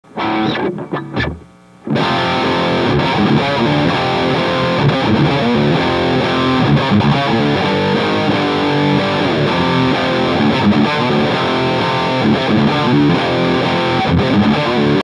Metal Demo: Peavy 6505MH & #4 Jim Root Orange Cabinet
6505® MH Mini Amp Head
Featuring an all tube preamp and power amp, the 6505 MH authentically produces the much loved tones of the legendary Peavey 6505 in a small, portable package.
The #4 Jim Root Signature PPC212 shares the same dimensions as the PPC212OB, but its closed-back construction and 12″ Voice of the World speakers give this cab a very different character.